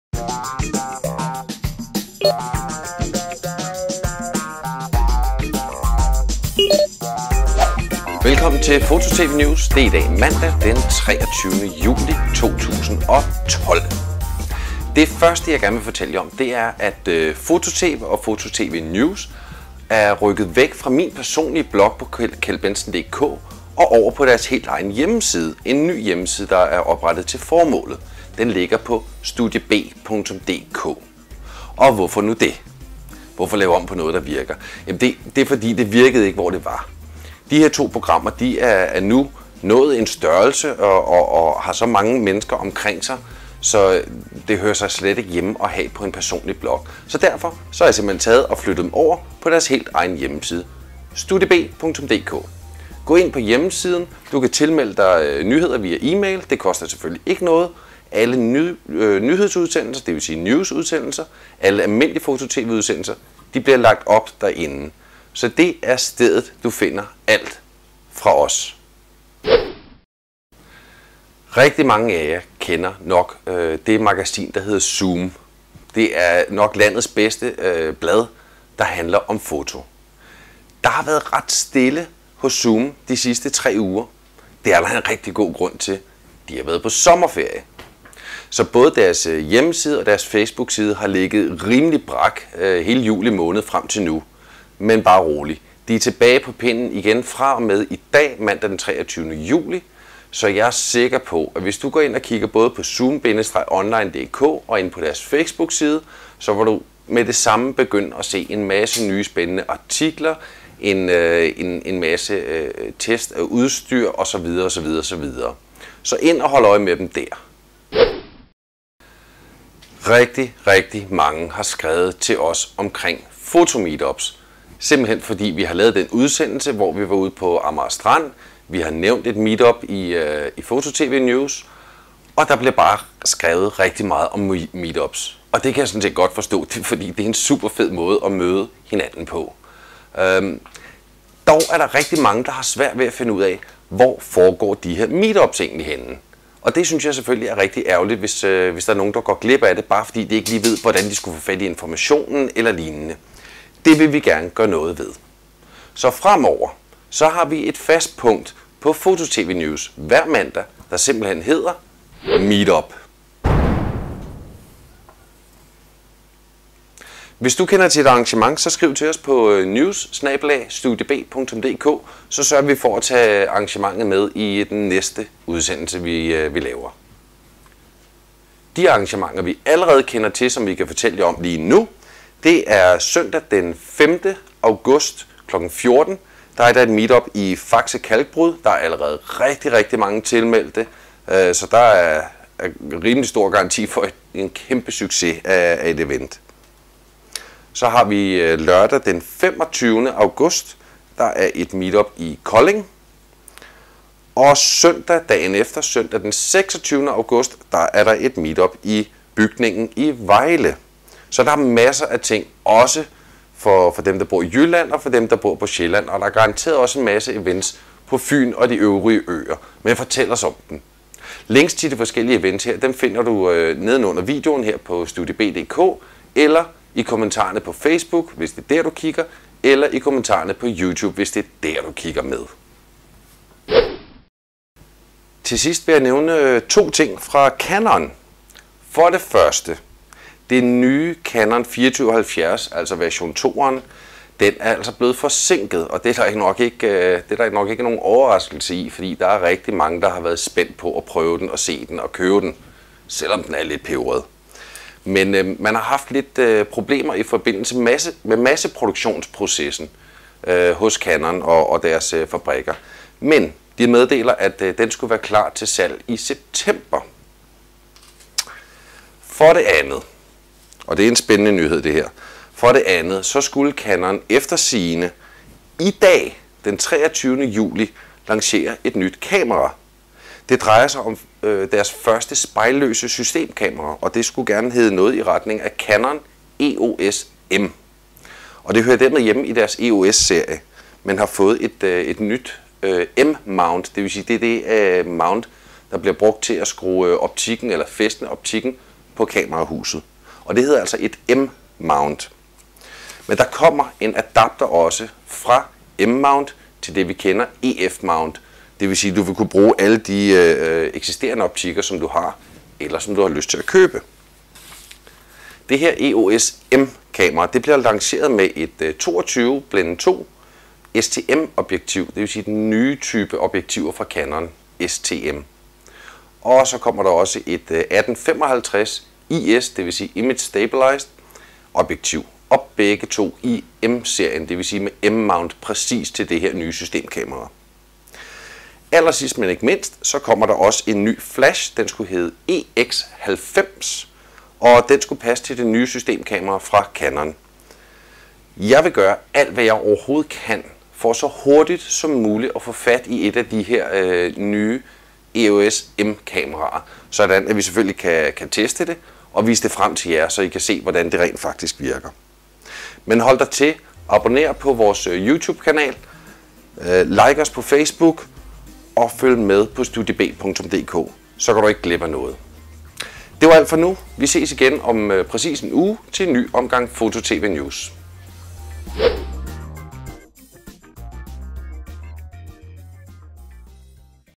FotoTVNEWS 23. juli 2012
Ugens nyheder den 23. juli 2012. Se bla Canon EOS M systemkameraet. Hør om kommende Photo Meet-Ups.

Se alle kommentarer og links på udsendelsens side hos StudieB

SHOW NOTES

Studieb.dk er nyt hjem for FotoTV og FotoTV NEWS

Se FotoTV 16 med reportage fra Meet Up på Amager Strandpark

Photo Meet Up Fakse Kalkbrud den 5. august 2012 kl. 14

MeRox Meet Up Geografisk Have i Kolding den 25. august kl. 10

Meet Up i Bygningen i Vejle den 26. august 2012 kl. 10